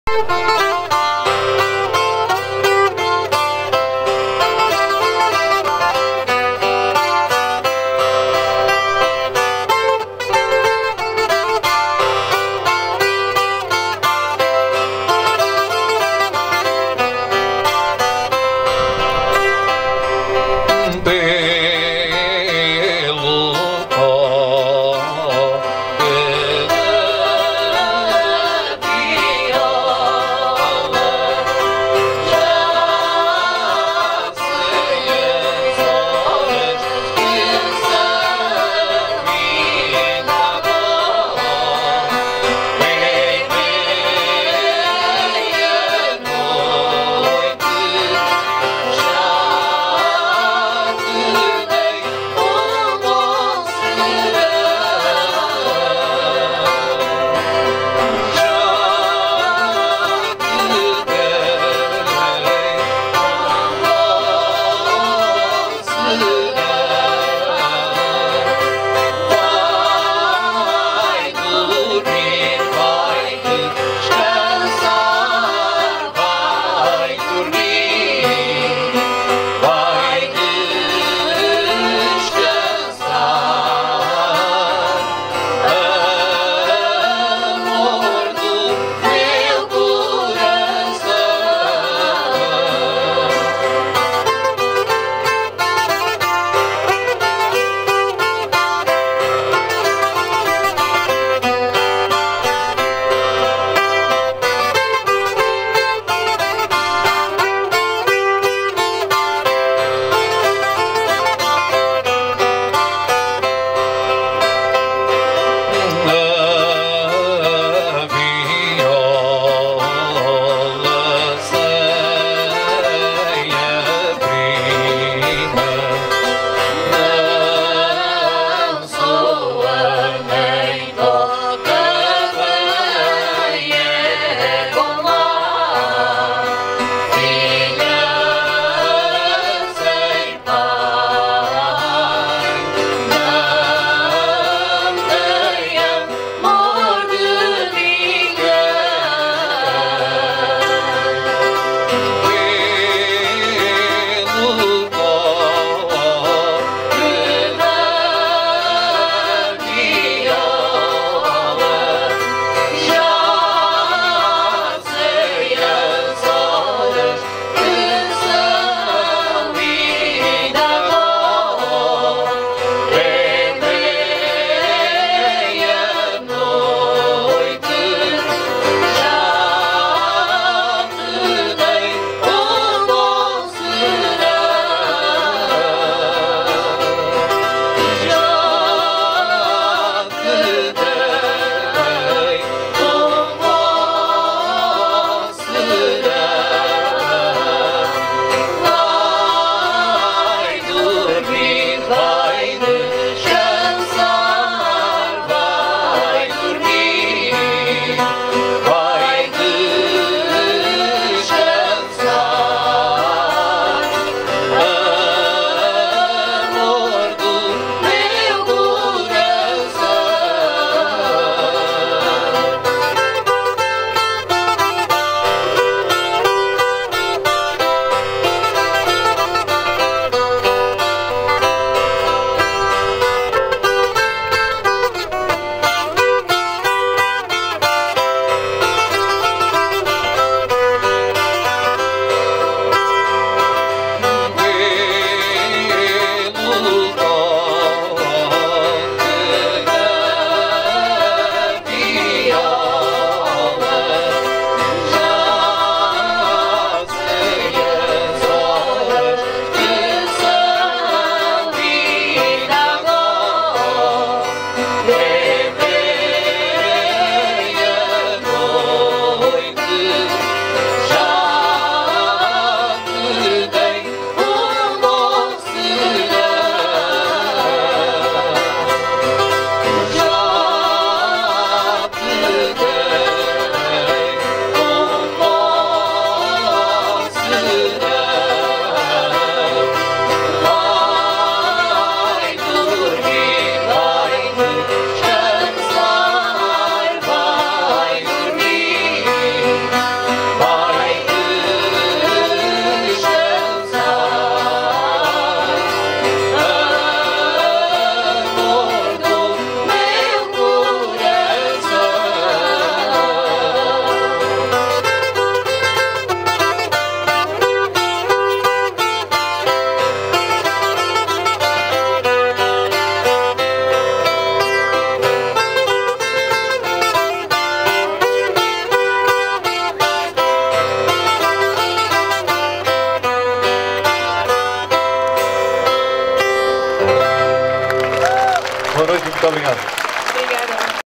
Violas Campaniças de Castro Verde
Encontro de Grupos Corais  Homenagem a José Luís Fragoso   Alvito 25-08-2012